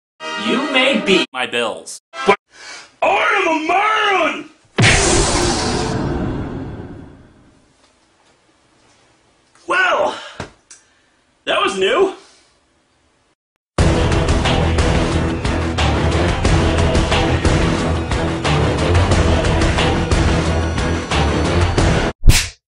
Linkara vs his bills
The prequel to Linkara's ad video. Can you really blame the guy?